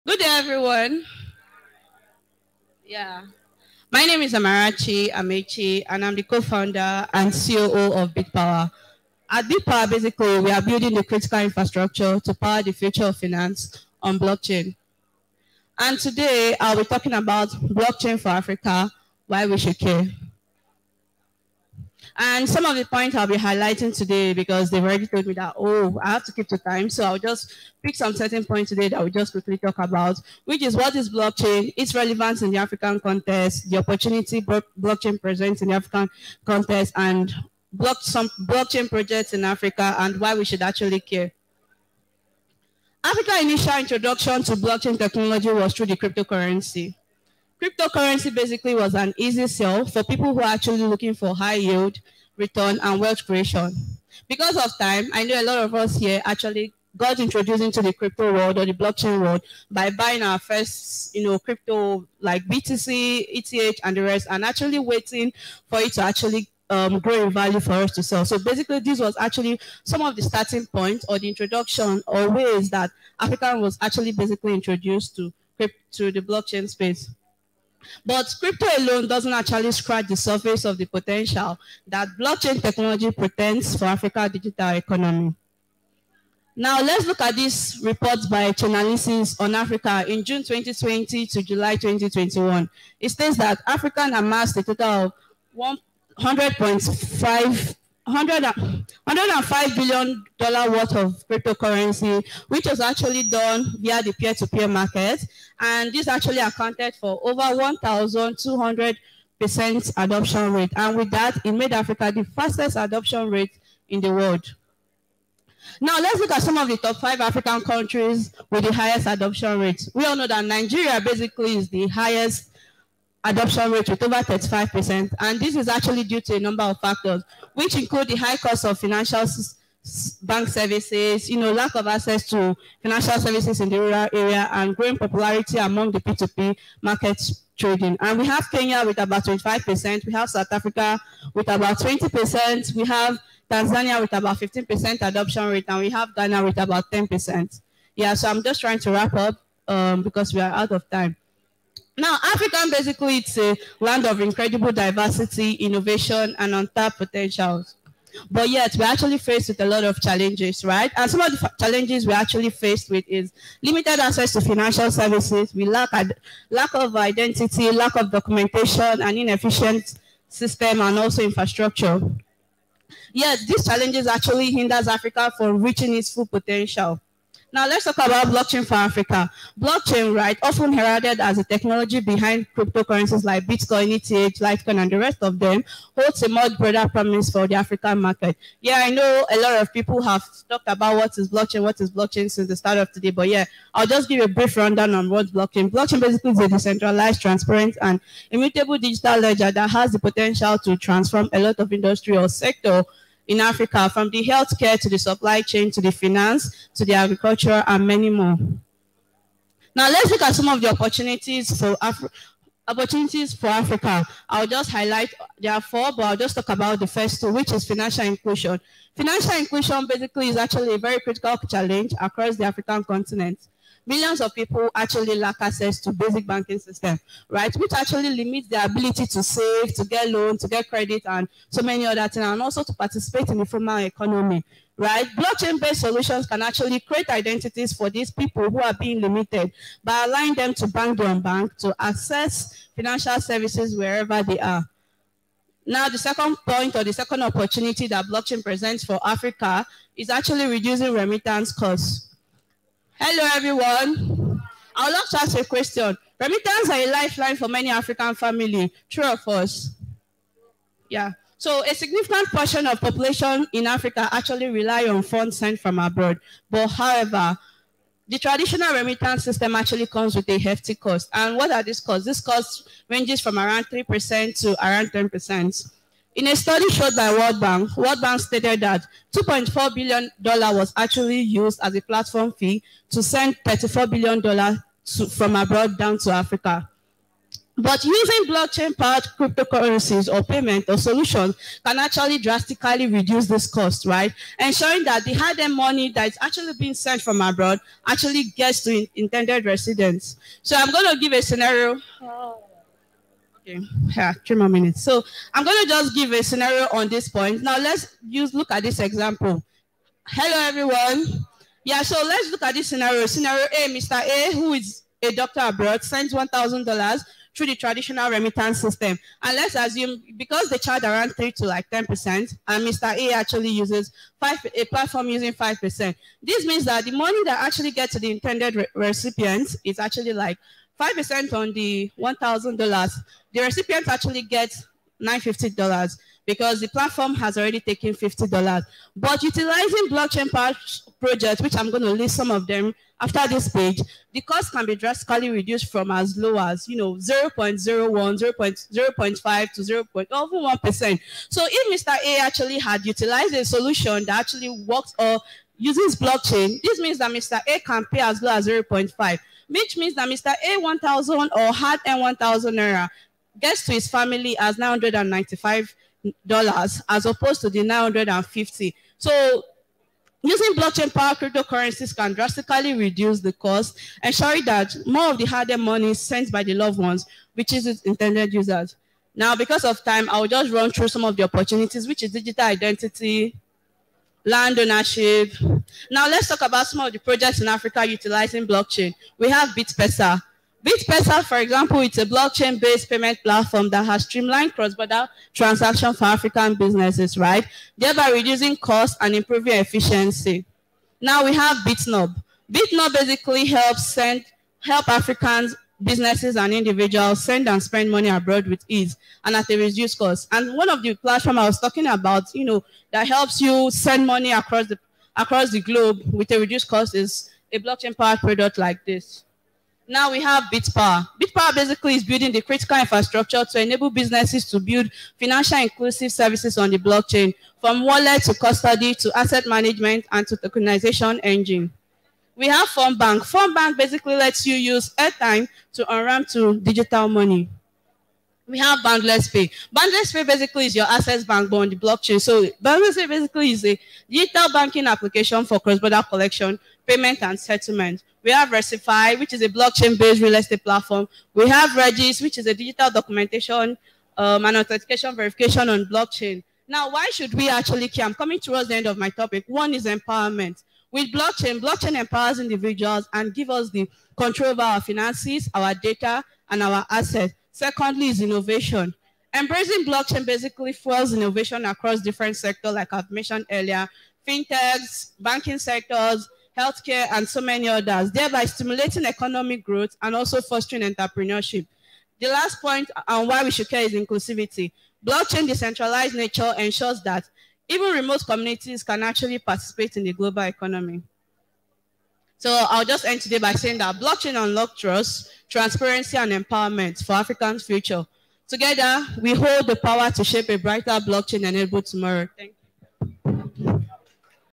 Blockchain for Africa: Why we should care - Amarachi Amaechi
Excerpt from Web3bridge Lagos Conference 2023
Speaker - Amarachi Amaechi
Topic - Blockchain for Africa: Why we should care